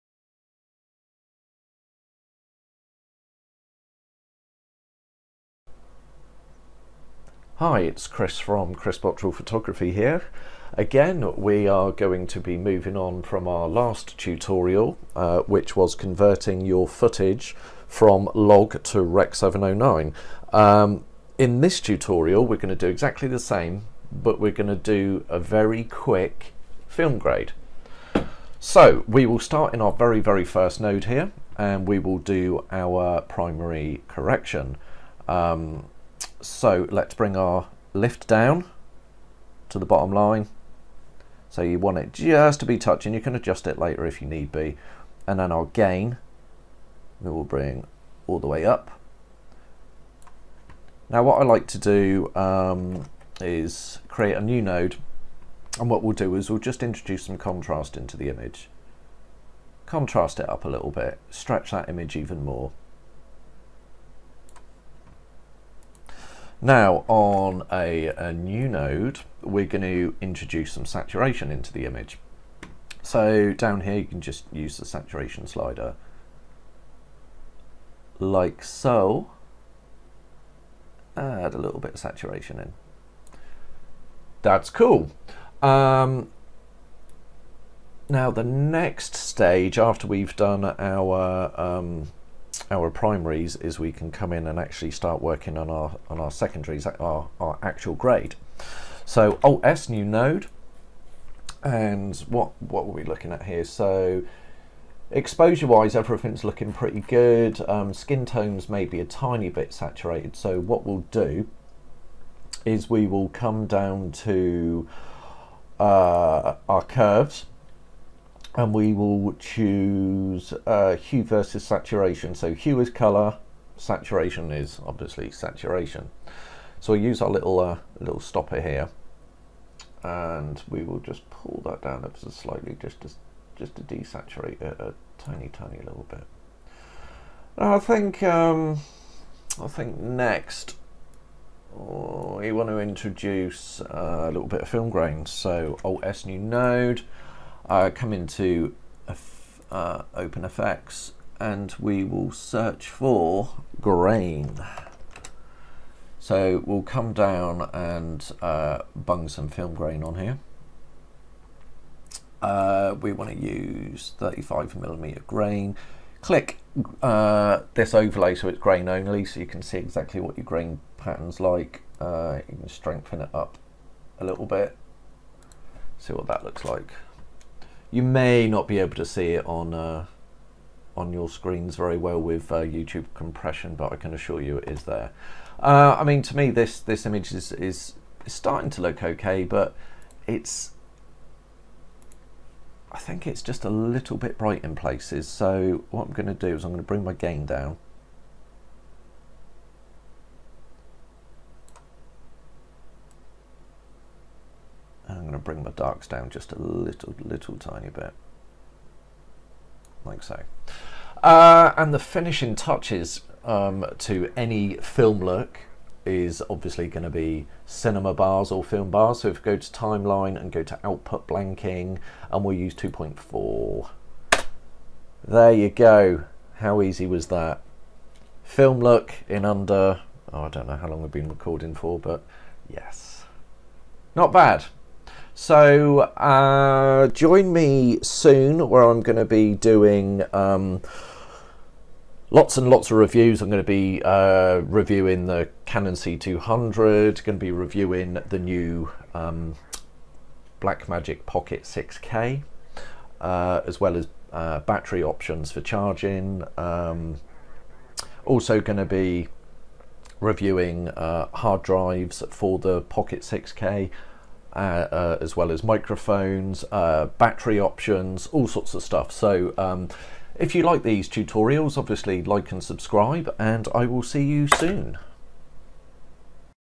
Davinci Resolve Film look from scratch
In this Davinci Resolve tutorial, we will take a LOG image and grade into a simple film look.